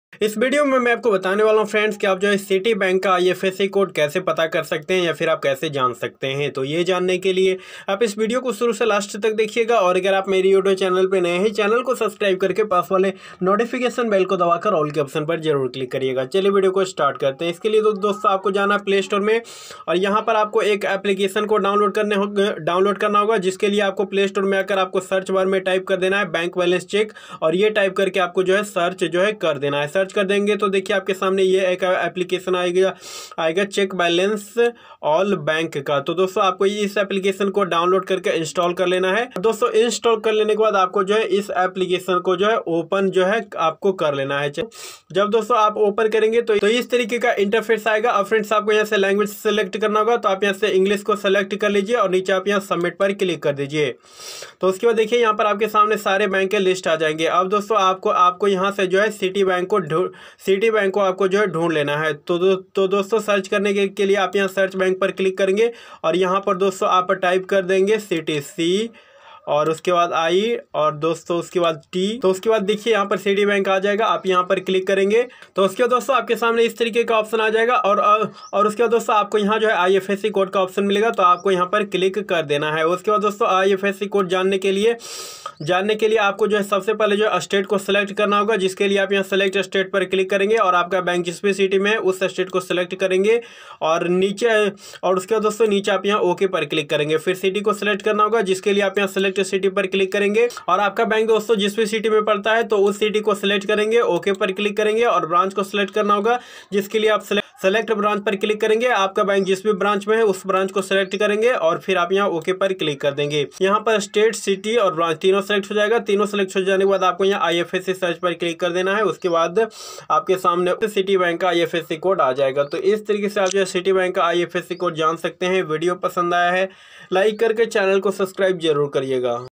Citi Bank IFSC code।। Citi Bank ka IFSC code kaise jane/nikale।। IFSC code of Citi Bank
is video me bataya gaya hai friends ki Citi Bank ka IFSC code Kaise Pata Kiya jata hai